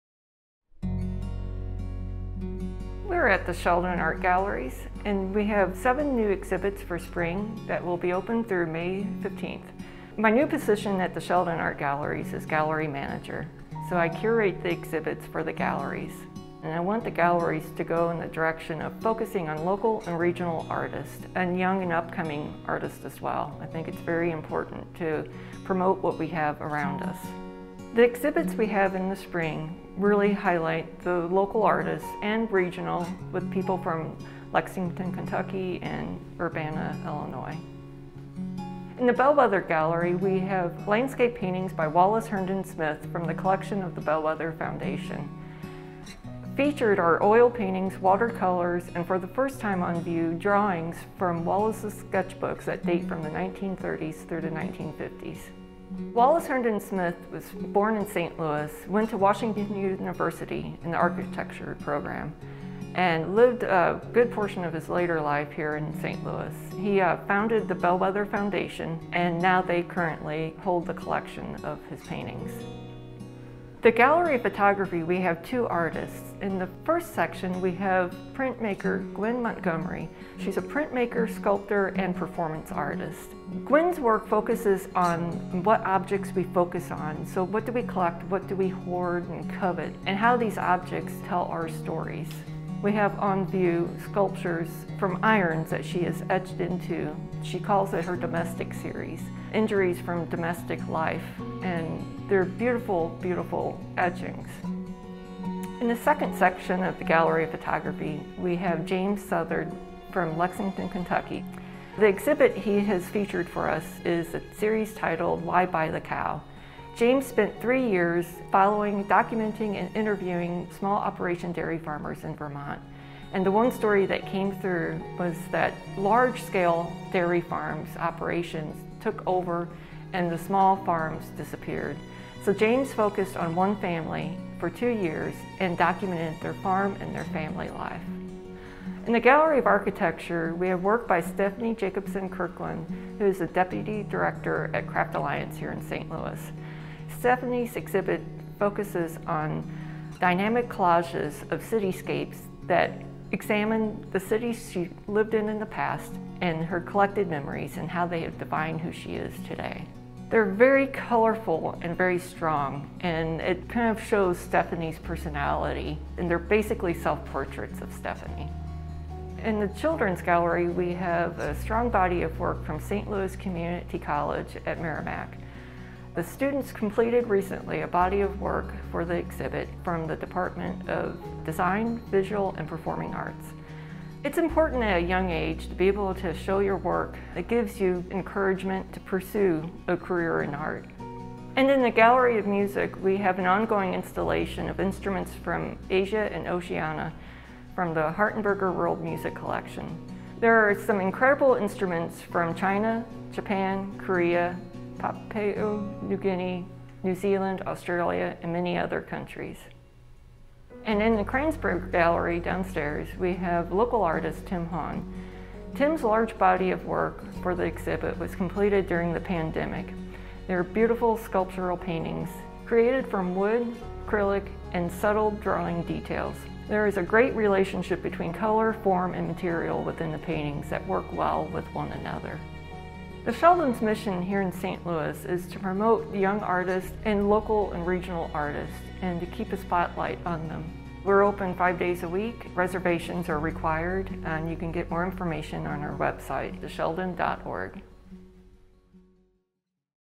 The Sheldon Art Galleries' New Spring 2021 Exhibits
The Sheldon Galleries are currently showcasing multiple new solo exhibits by artists in the St. Louis region. Featured are a collection of landscape paintings by Wallace Herndon Smith, the prints and sculptural work of Guen Montomery, dynamic cityscape collages by Stefanie Jacobson Kirkland, sculptural paintings by Tim Hahn, and the photography of James R. Southard. Also on show are various pieces by students from St. Louis Community College’s Meramec campus and a collection of stunning instruments from the Far East and Oceania.
These exhibits will continue through May 15, 2020 at the Sheldon. for more information.